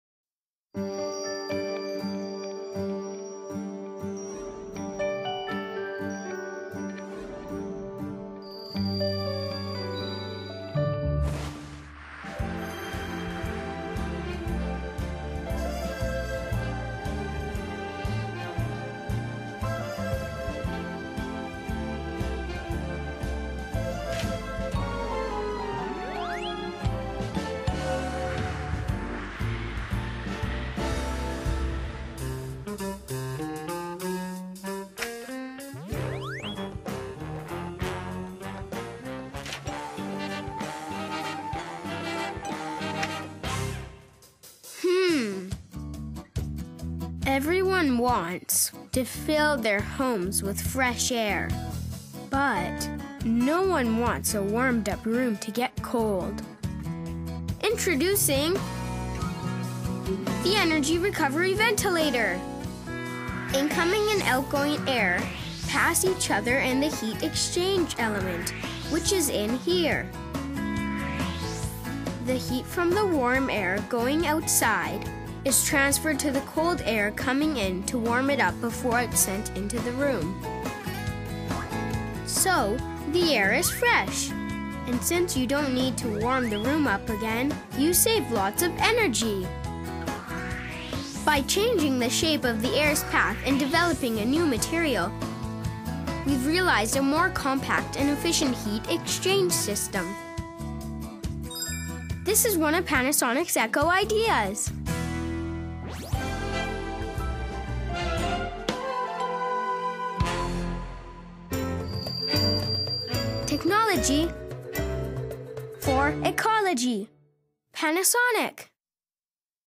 Eco Ideas - Technology for Heat Exchange
Eco Ideas kali ini, heat exchange. Membahas bagaimana teknologi Panasonic berperan dalam menghemat energi.